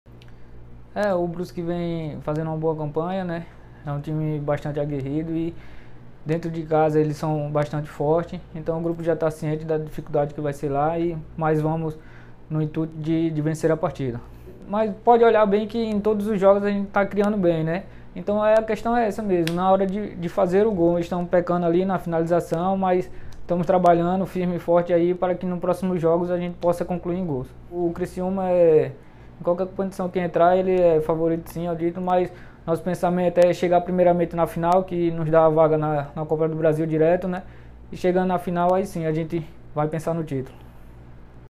Meia Ítalo Melo fala sobre a preparação do Tigre para a partida diante do Brusque
Vídeo: TV Tigre